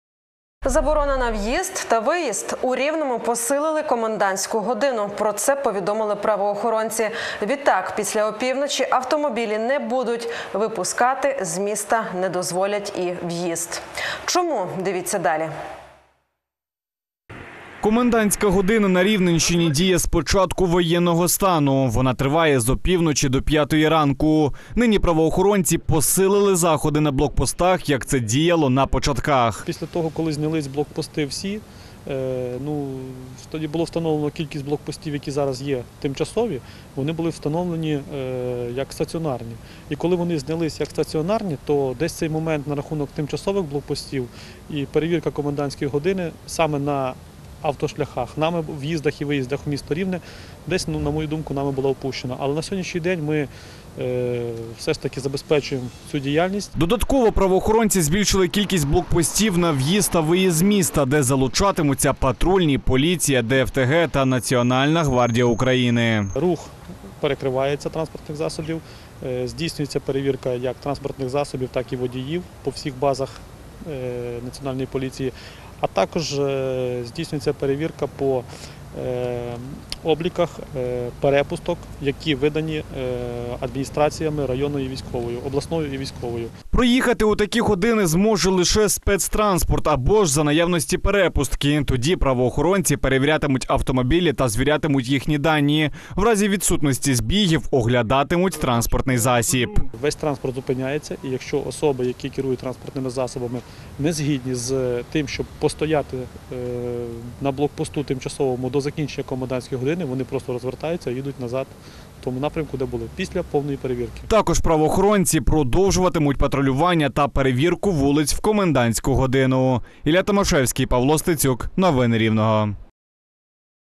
Чому та як у Рівному посилюють комендантську годину?
Заборона на в'їзд та виїзд: у Рівному посилили комендантську годину. Про це повідомили правоохоронці. Відтак, після опівночі, автомобілі не будуть випускати з міста. Не дозволять і в'їзд. Чому - дивіться далі.